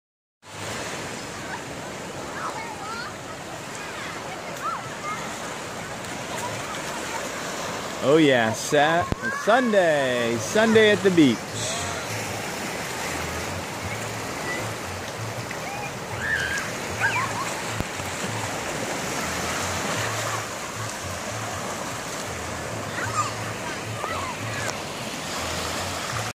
Tread Lightly Kauai videos
Anini beach just jumping on a day hotter that the 4th of july! It's Sunday and that means funday for moms and kids.

Various videos taken in the Hawaiian islands during tour and vacation fun by Jay at Tread Lightly Kauai. Coming to Kauai? or google search Tread Lightly Kauai Tours.